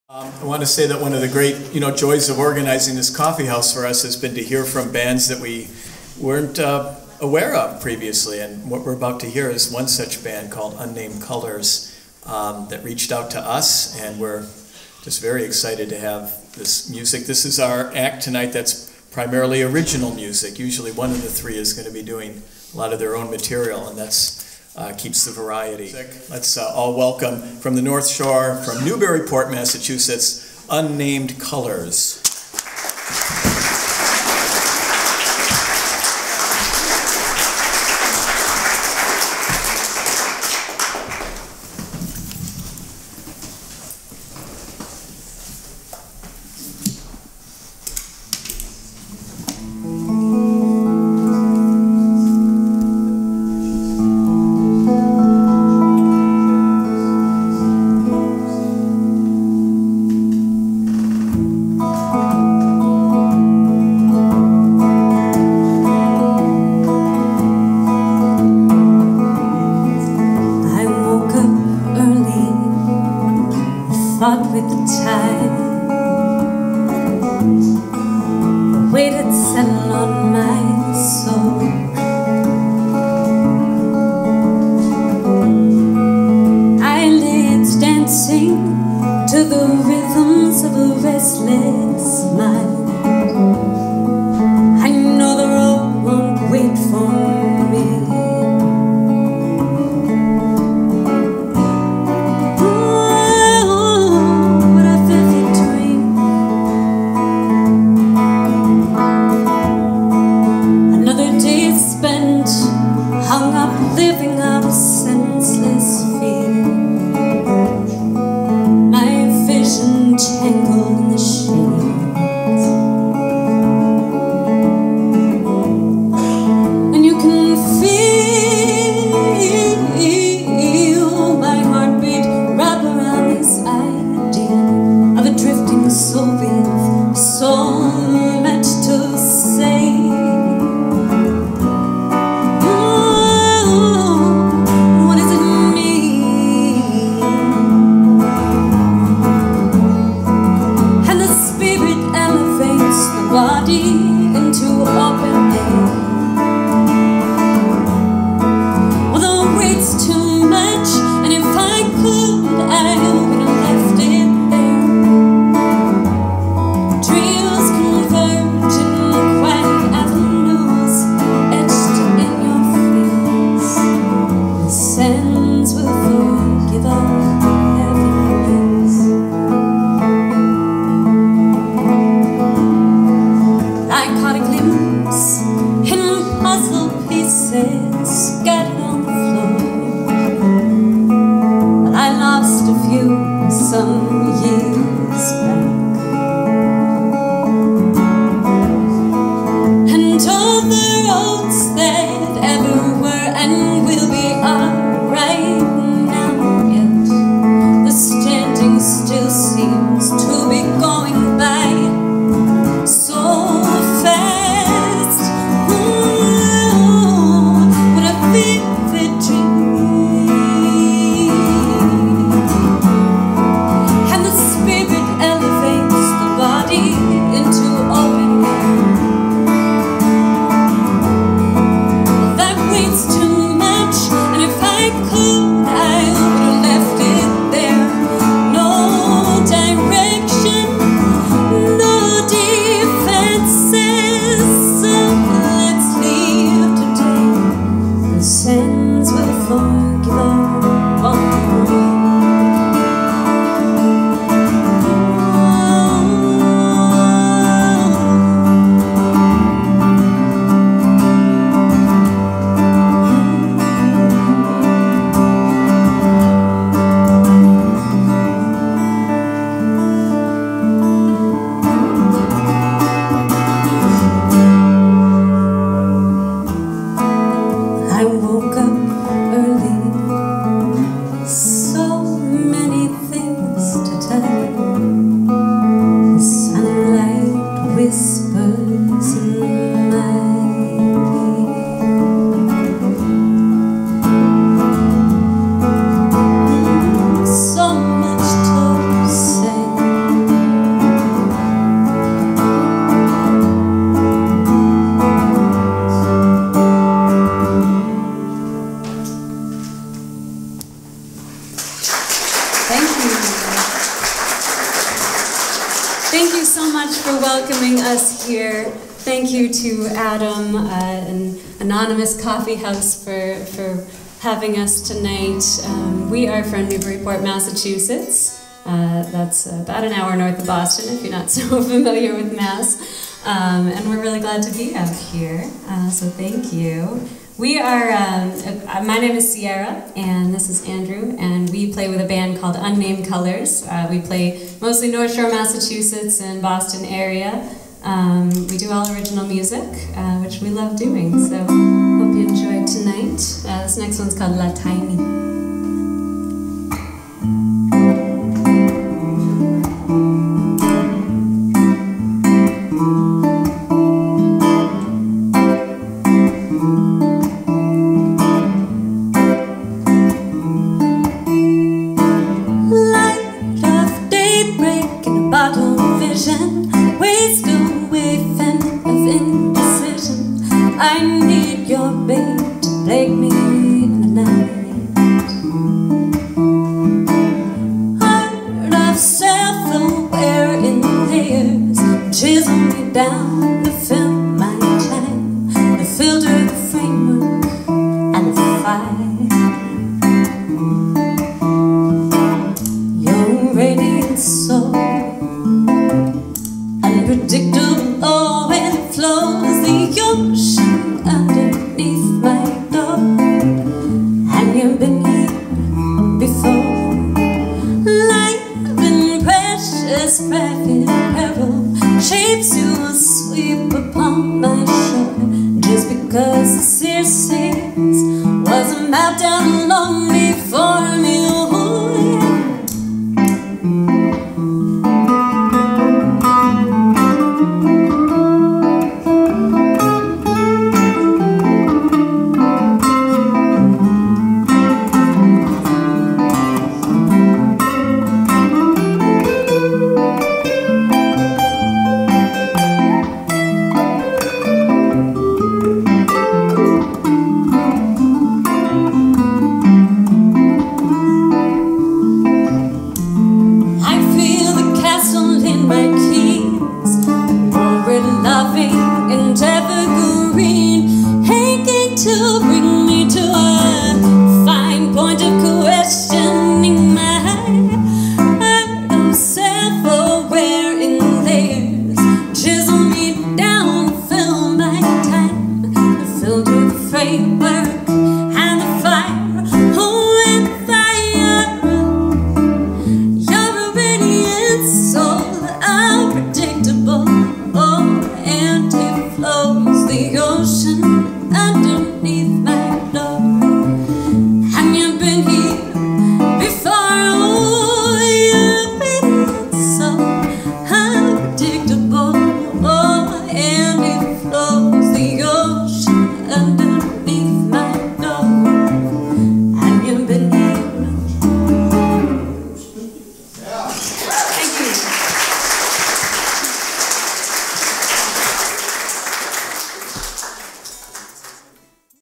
UNAMED COLORS at the Anonymous Coffeehouse in Lebanon NH on January 17, 2020
Music in the Upper Connecticut River Valley of New Hampshire and Vermont recorded by Chad Finer of Norwich, Vermont